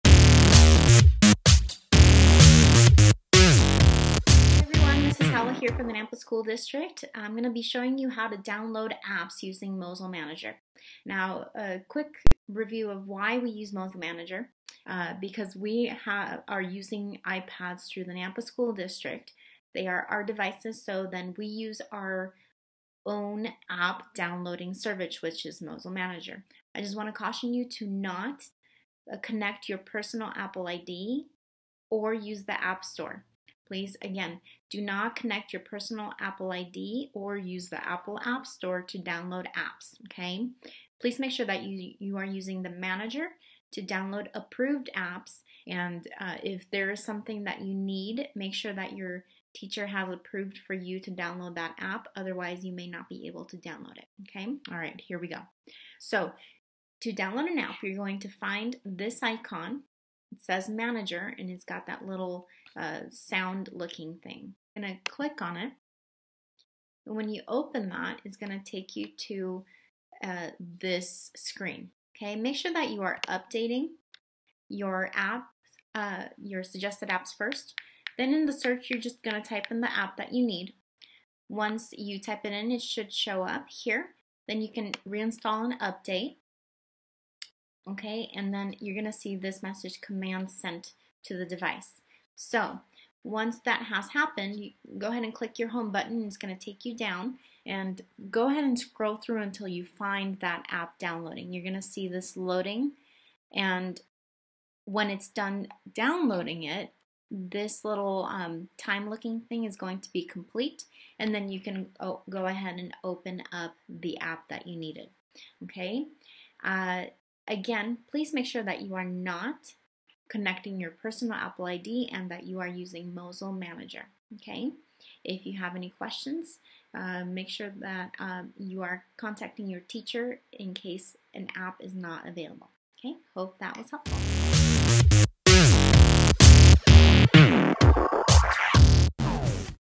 How to Download Apps using Mosyle Manager
This tutorial is to help parents and students in the Nampa School District #131 download approved apps on their devices through the Mosyle Manager Service. Parents and students, please do not download apps using your personal Apple ID or the Apple App Store.